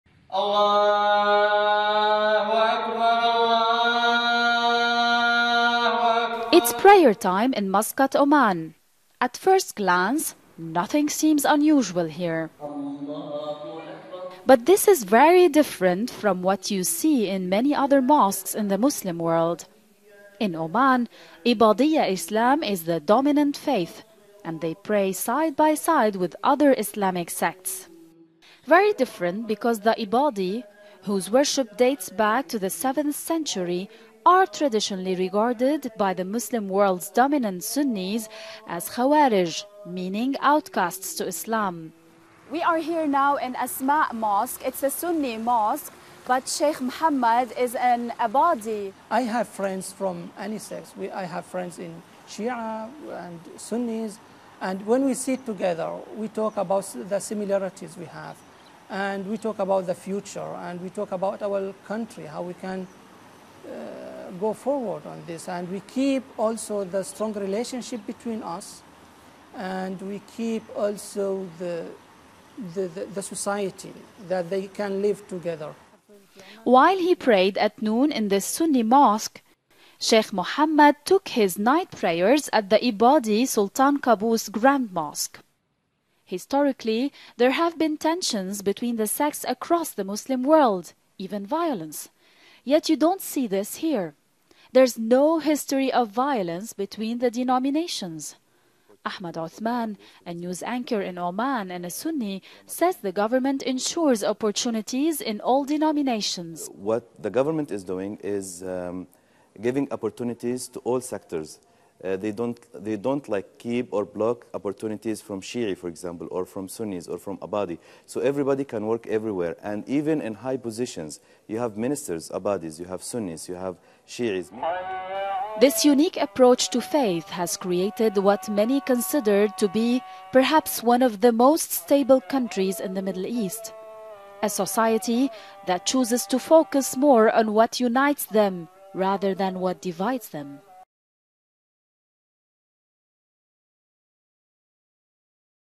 CNN's Rima Maktabi discovers religious unity among diverse Islamic sects in Oman..mp4
This is the best example of united and tolerant muslims !